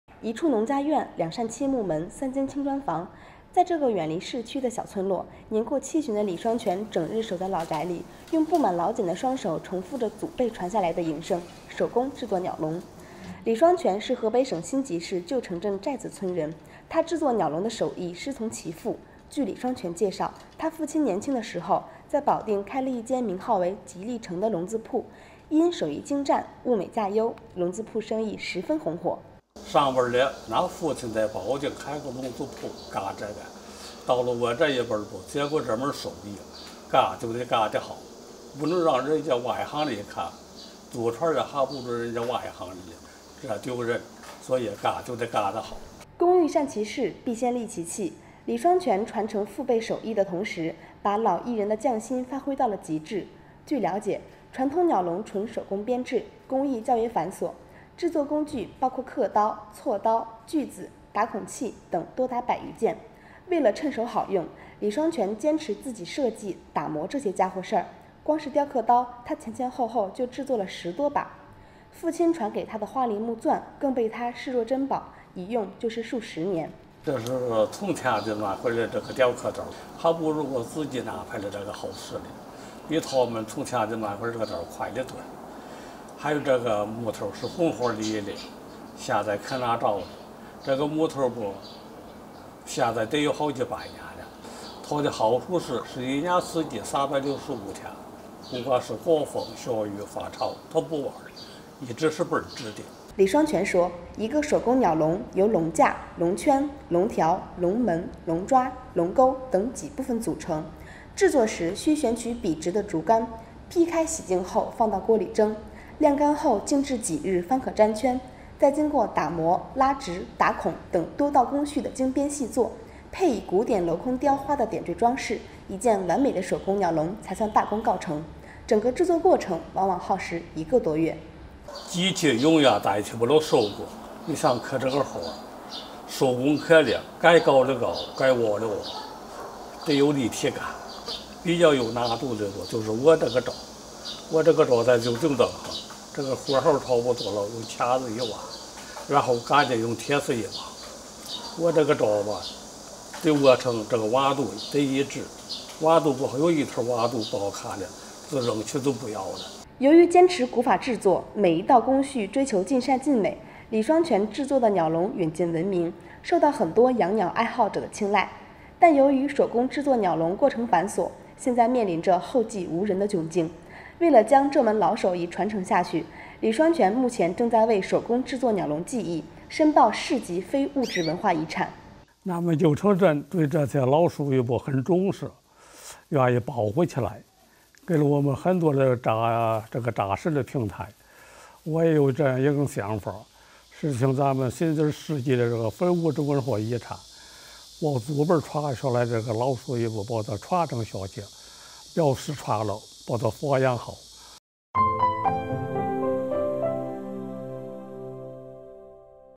“守”艺人李双全：指尖传古法 匠心筑鸟笼
一处农家院、两扇漆木门、三间青砖房……在这个远离市区的小村落，年过七旬的李双全整日守在老宅里，用布满老茧的双手重复着祖辈传下来的“营生”——手工制作鸟笼。
李双全是河北省辛集市旧城镇寨子村人，他制作鸟笼的手艺师从其父。据李双全介绍，他父亲年轻的时候，在保定开了一间名号为“吉立成”的笼子铺，因手艺精湛、物美价优，笼子铺生意十分红火。